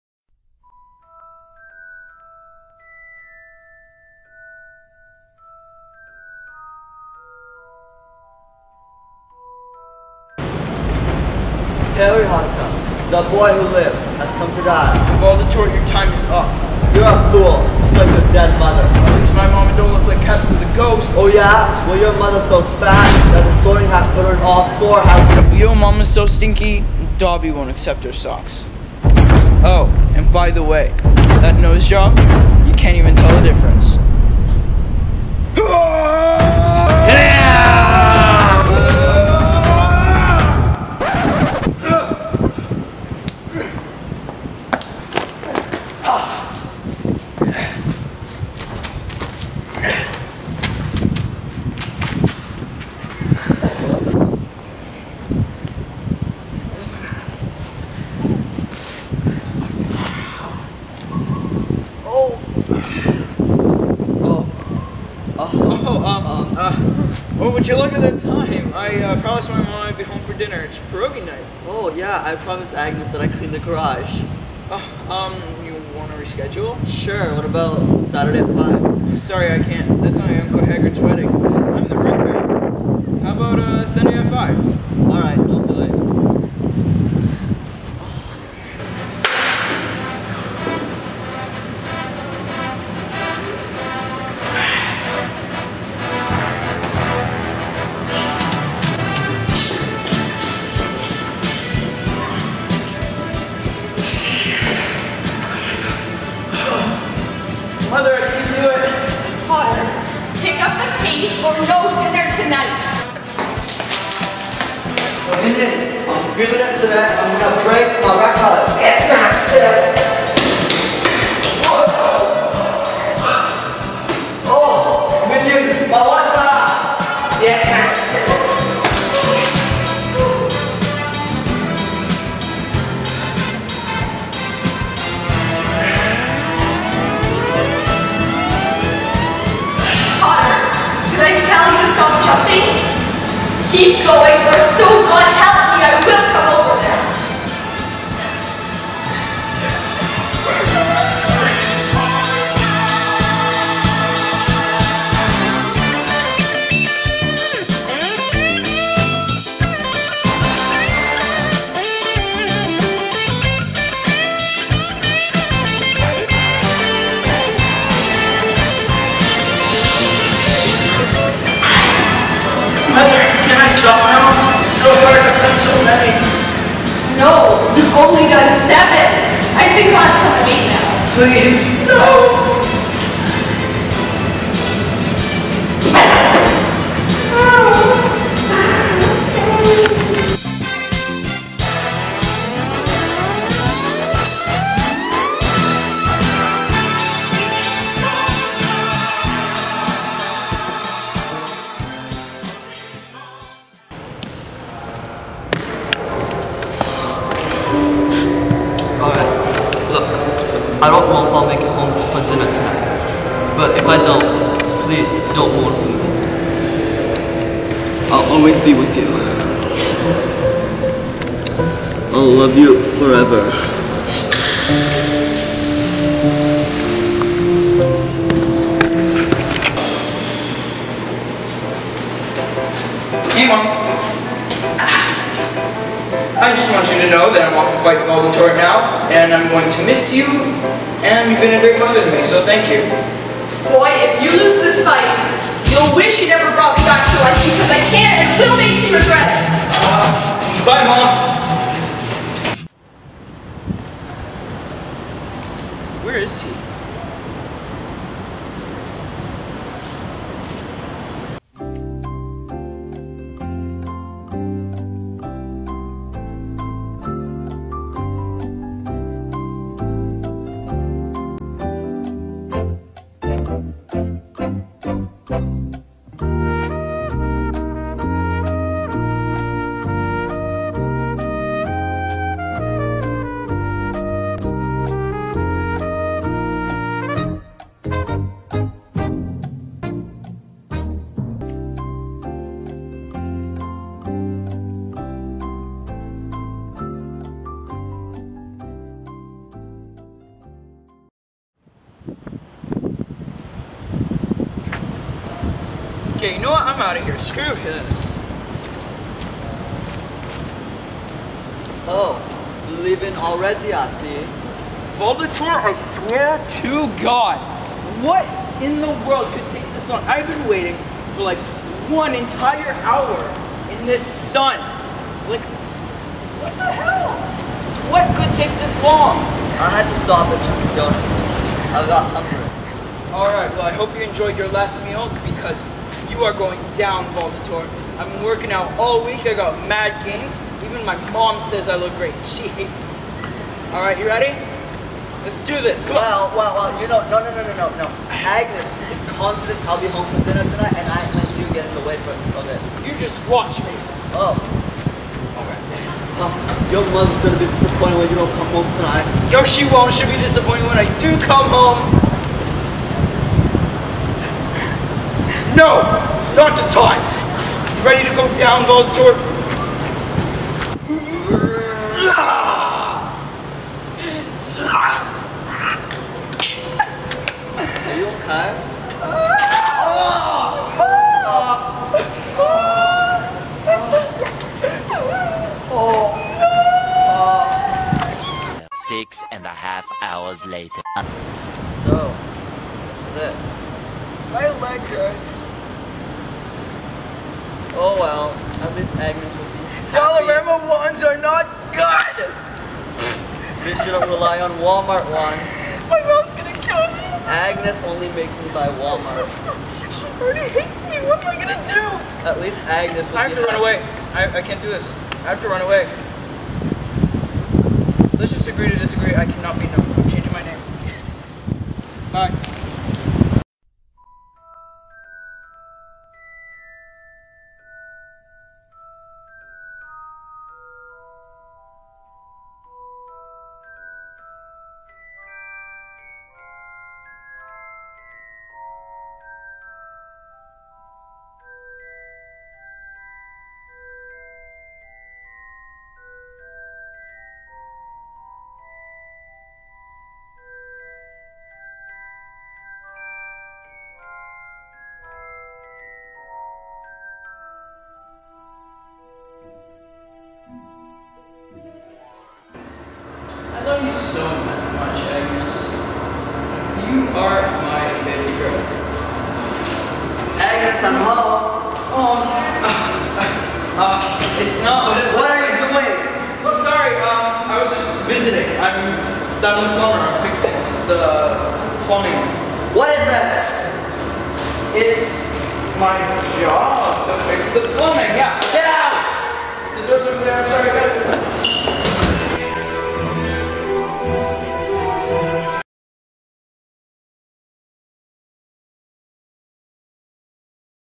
Parry Hotter and the courtyard combat
A intense short film filled with love, action, drama, and motherly love